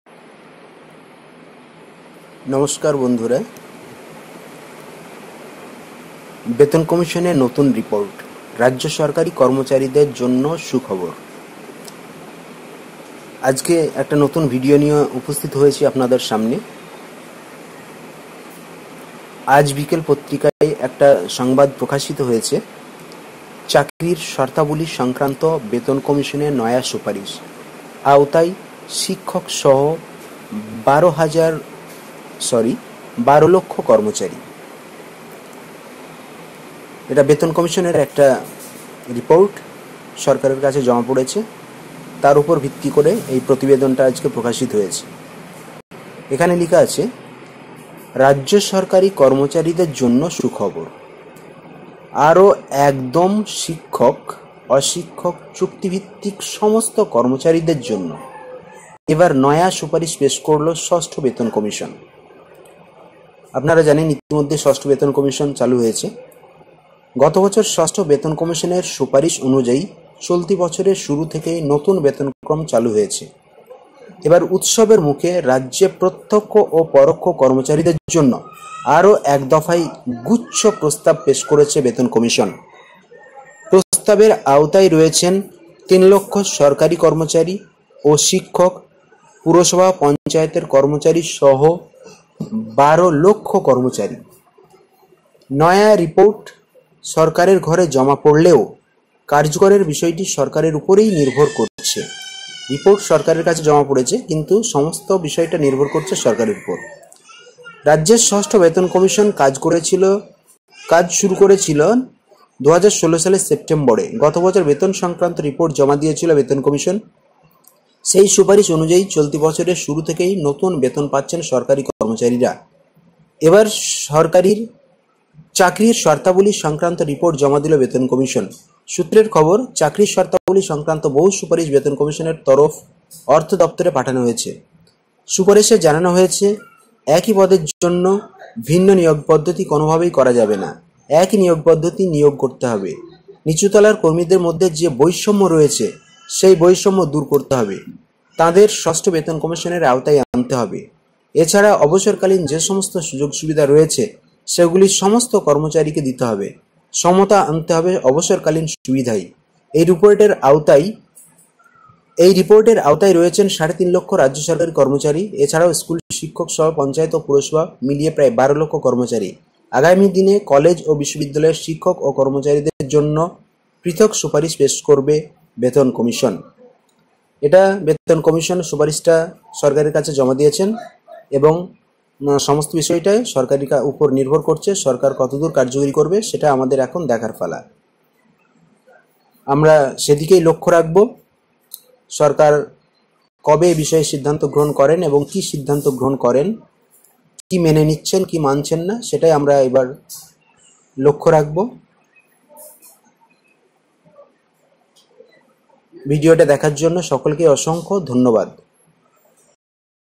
রাজ্য সরকারি কর্মচারীদের জন্য সুখবর.......
বেতন কমিশনের নতুন রিপোর্ট। রাজ্য সরকারি কর্মচারীদের জন্য সুখবর।